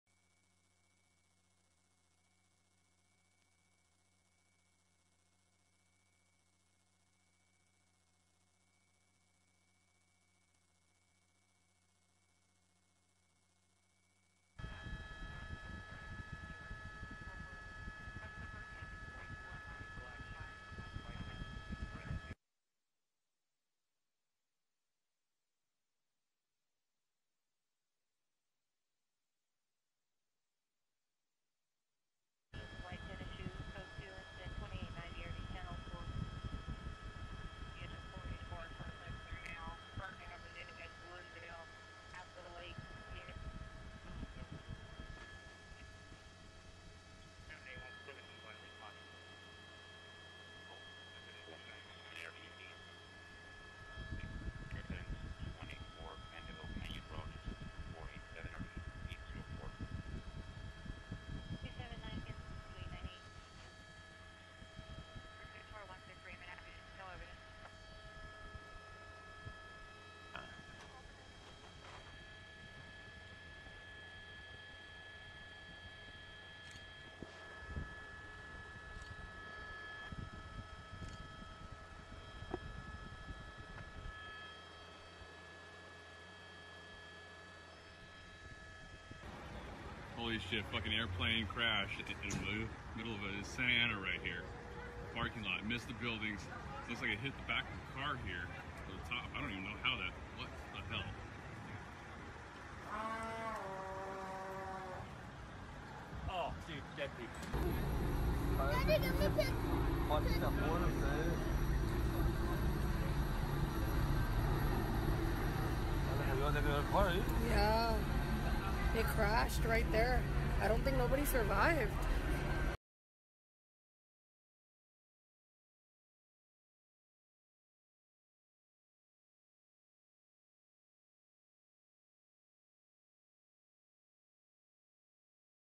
美 LA 인근서 경비행기 추락…탑승자 5명 전원 사망 / 연합뉴스 (Yonhapnews)
미국 캘리포니아 주 로스앤젤레스(LA) 남쪽 오렌지카운티 샌타애나의 한 주차장에 경비행기가 추락해 탑승자 5명이 모두 사망했다고 미 연방항공청(FAA)이 6일(현지시간) 밝혔다.